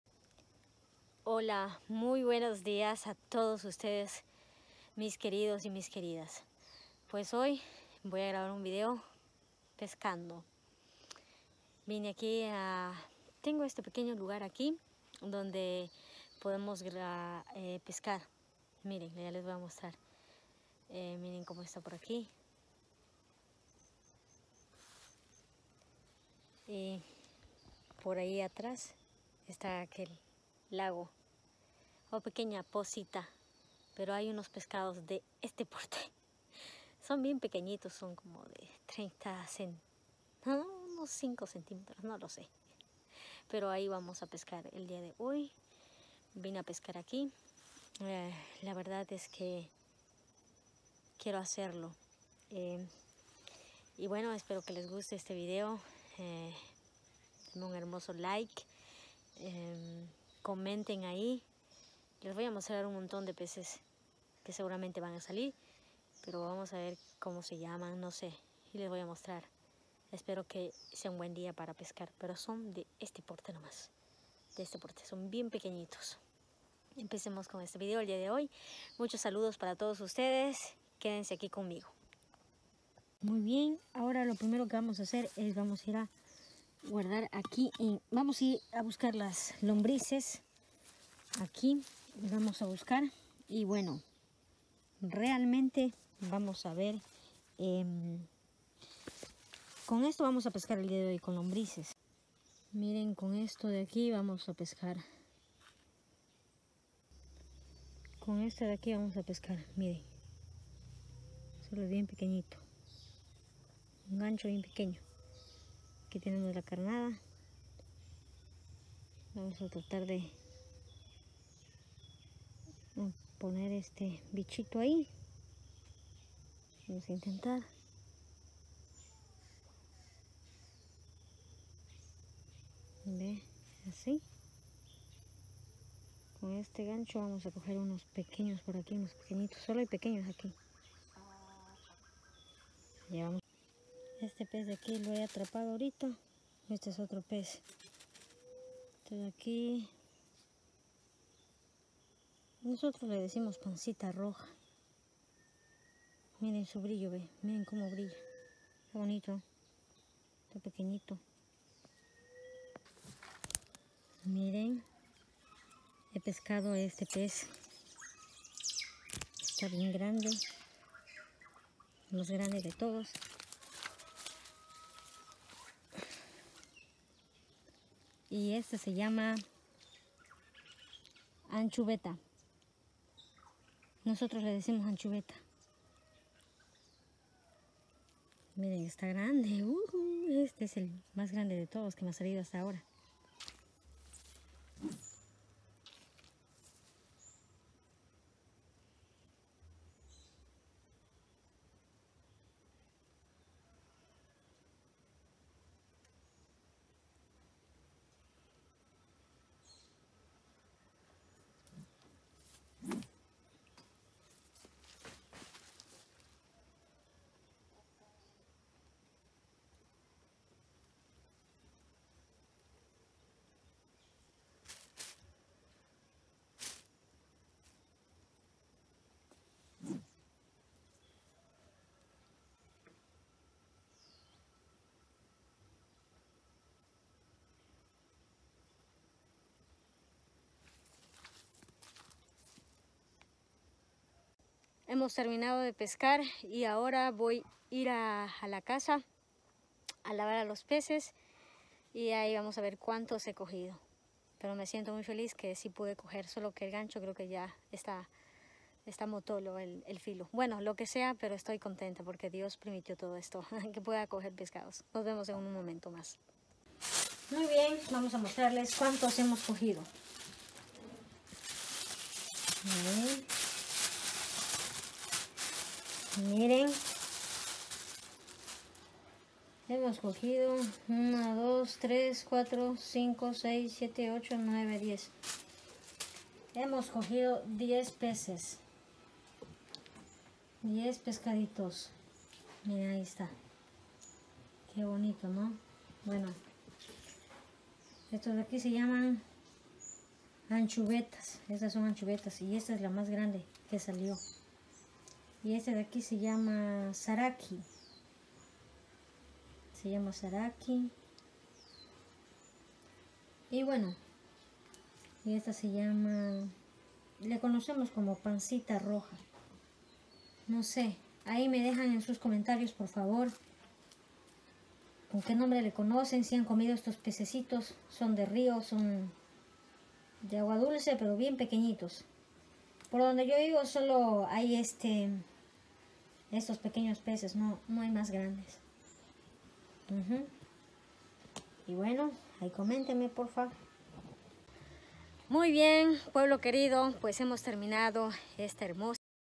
PESCANDO PECES EN LA FINCA. ME GUSTA ESTO. LA VIDA Y LA NATURALEZA.
AQUI ME FUI DE PESCAR EN LA FINCA. SALUDOS A TODOS BENDICIONES.